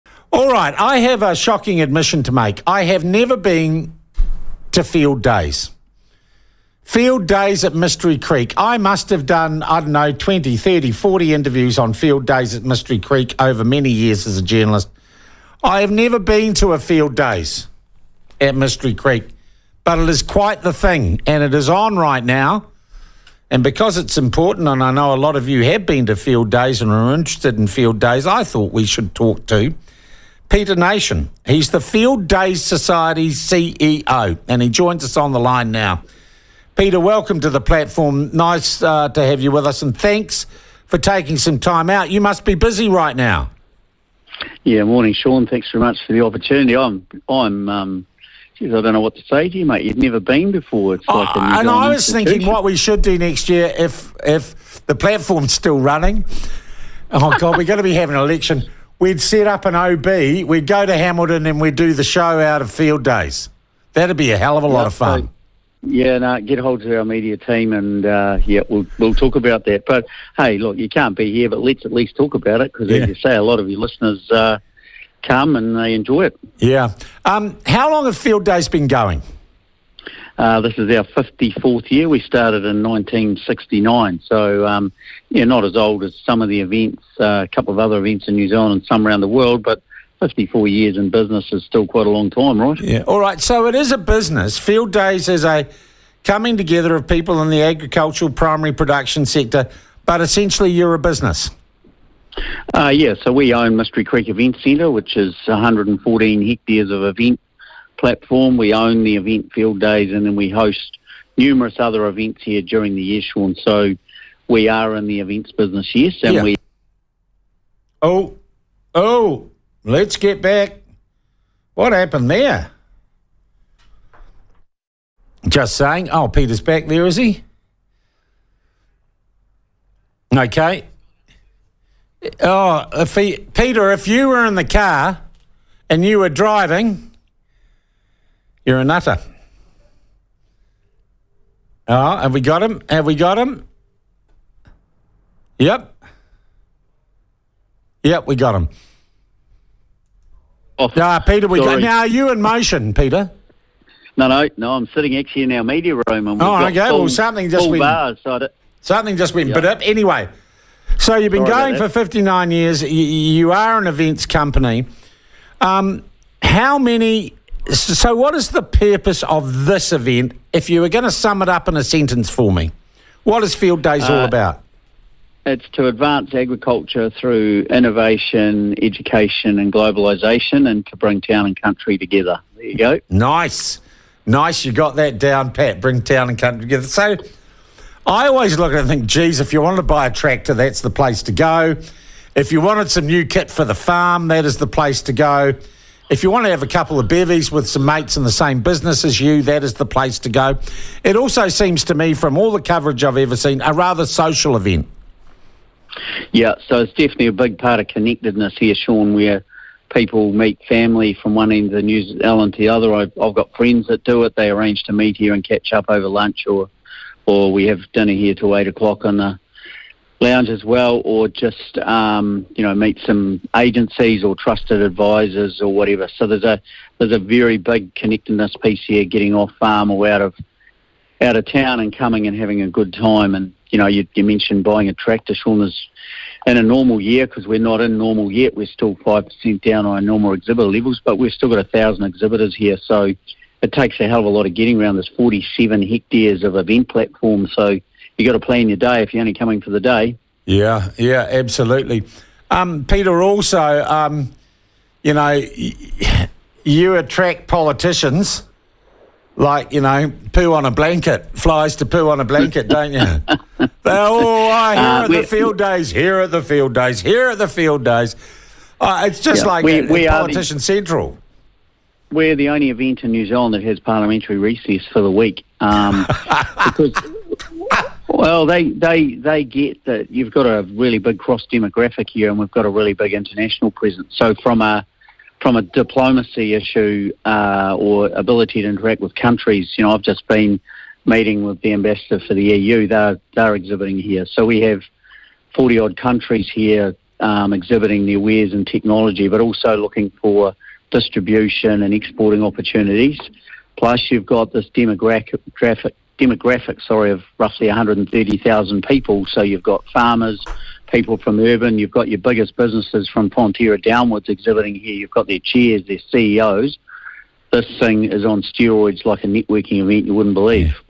Peter Nation on Fieldays 2022
NZ National Fieldays Society CEO Peter Nation talks to Sean Plunket about Fieldays

Sean Plunket: 7am - 10am
Michael Laws: 10am - 1pm
Martin Devlin: 1pm - 4pm